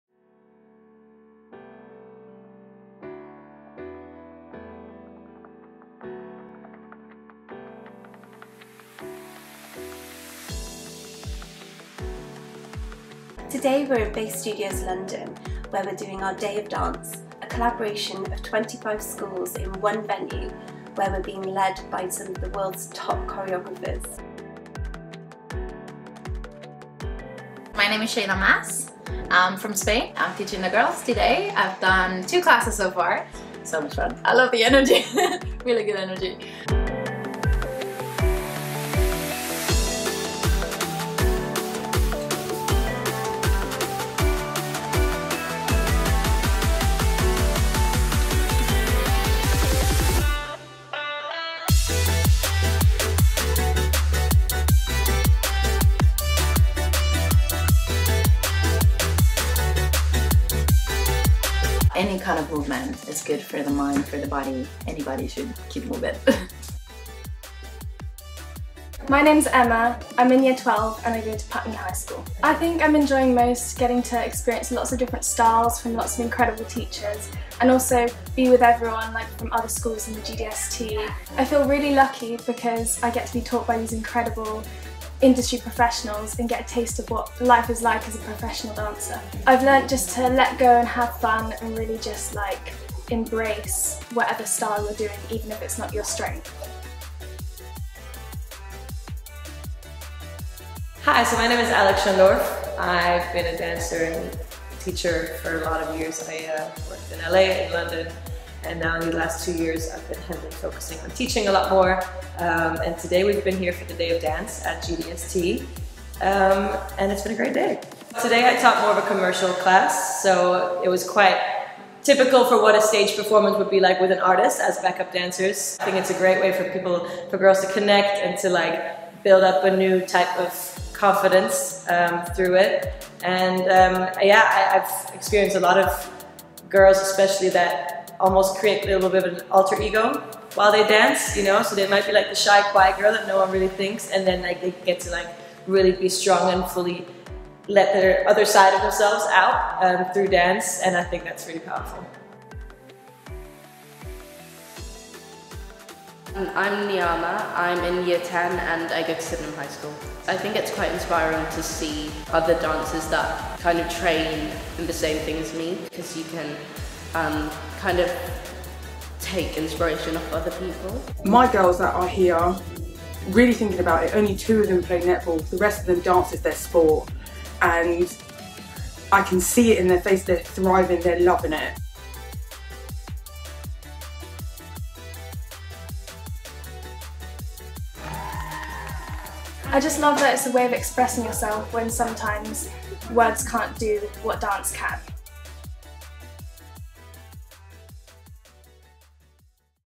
Day of Dance 2021
150 girls from 21 schools across the GDST family came together in London on 6th December 2021 to participate in the annual Day of Dance, held at the iconic BASE Studio in London’s Vauxhall. Covid prevented this gathering from happening in 2020, and students were delighted to meet in person again to share and collaborate with some of the world’s top choreographers in dance workshops in styles ranging from musical theatre, to contemporary, to commercial.